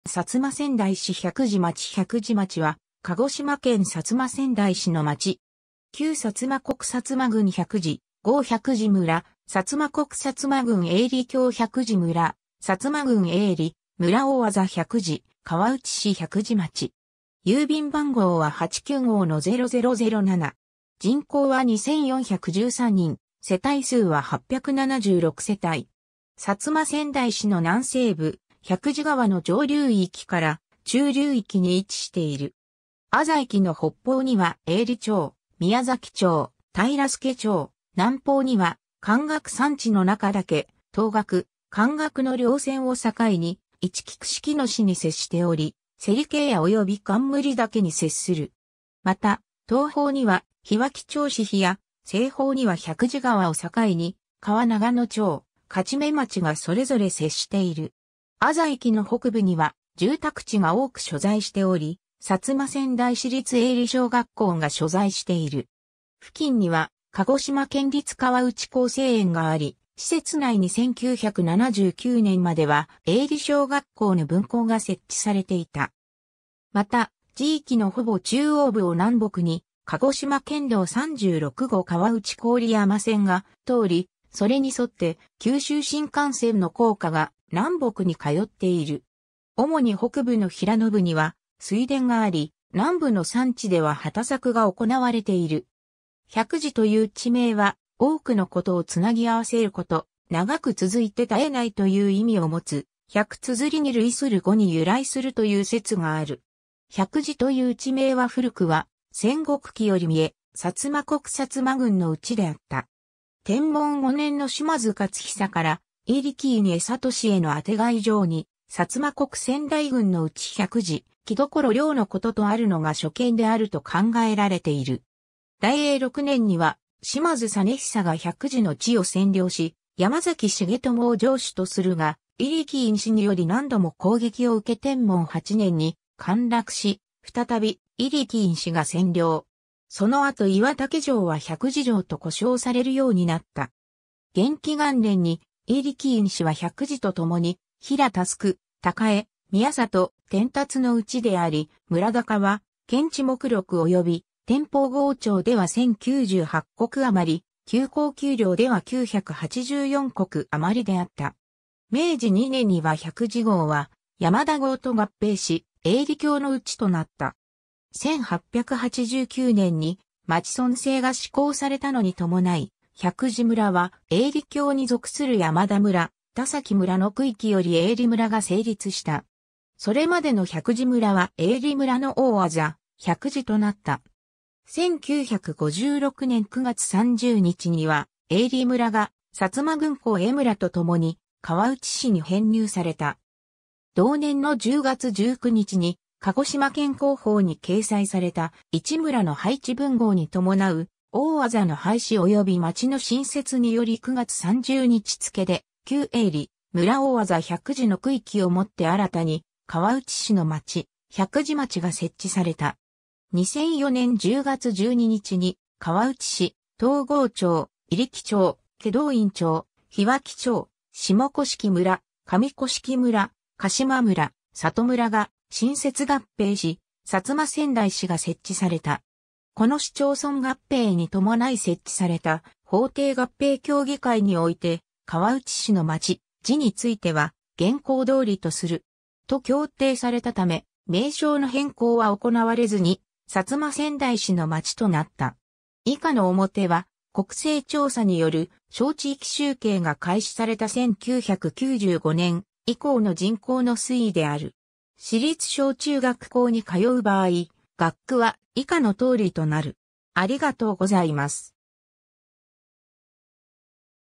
百次町
薩摩川内市の町・字
日本 」 鹿児島県 」 薩摩川内市 」 百次町 百次町（ももつぎちょう）は、鹿児島県薩摩川内市の町。
旧薩摩国薩摩郡百次郷百次村、薩摩国薩摩郡永利郷百次村、薩摩郡永利村大字百次、川内市百次町。
 薩摩川内市の南西部、百次川の上流域から中流域に位置している。
字域の北方には永利町、宮崎町、平佐町、南方には冠岳山地の中岳、東岳、冠岳（西岳）の稜線を境にいちき串木野市に接しており、芹ケ野及び冠嶽に接する。
また、東方には樋脇町市比野、西方には百次川を境に川永野町、勝目町がそれぞれ接している。
 字域の北部には住宅地が多く所在しており、薩摩川内市立永利小学校が所在している。
付近には鹿児島県立川内厚生園があり、施設内に1979年までは永利小学校の分校が設置されていた。
 また、字域のほぼ中央部を南北に鹿児島県道36号川内郡山線が通り、それに沿って九州新幹線の高架が南北に通っている。
 主に北部の平野部には水田があり、南部の山地では畑作が行われている。
 「百次」という地名は多くのことをつなぎ合わせること、長く続いて絶えないという意味を持つ「百綴」に類する語に由来するという説がある。
 百次という地名は古くは戦国期より見え、薩摩国薩摩郡のうちであった。
天文5年の島津勝久から入来院重聡への宛行状に「薩摩国千台郡之内百次城所領之事」とあるのが初見であると考えられている。
 大永6年には島津実久が百次の地を占領し、山崎成智を城主とするが、入来院氏により何度も攻撃を受け天文8年に陥落し、再び入来院氏が占領。
その後岩田ケ城は百次城と呼称されるようになった。
元亀元年に入来院氏は百次と共に平佐（現在の平佐町）、高江（現在の高江町）、宮里（現在の宮里町）、天辰（現在の天辰町)、碇山を島津氏に献納した。 江戸期には薩摩国百次郷（外城）のうちであり、村高は「検地目録」及び「天保郷帳」では1,098石余、「旧高旧領」では984石余であった。
明治2年には百次郷は山田郷と合併し、永利郷のうちとなった。
 1889年（明治22年）に町村制が施行されたのに伴い、百次村は永利郷に属する山田村、田崎村の区域より永利村が成立した。
それまでの百次村は永利村の大字「百次」となった。
 1956年（昭和31年）9月30日には永利村が薩摩郡高江村と共に川内市に編入された。
同年の10月19日に鹿児島県公報に掲載された「 市村の廃置分合に伴う大字の廃止及び町の新設」（鹿児島県告示）により9月30日付で旧永利村大字百次の区域を以て新たに川内市の町「百次町」が設置された。
 2004年（平成16年）10月12日に川内市、東郷町、入来町、祁答院町、樋脇町、下甑村、上甑村、鹿島村、里村が新設合併し薩摩川内市が設置された。
この市町村合併に伴い設置された法定合併協議会において川内市の町・字については「現行通りとする。」と協定されたため、名称の変更は行われずに薩摩川内市の町となった。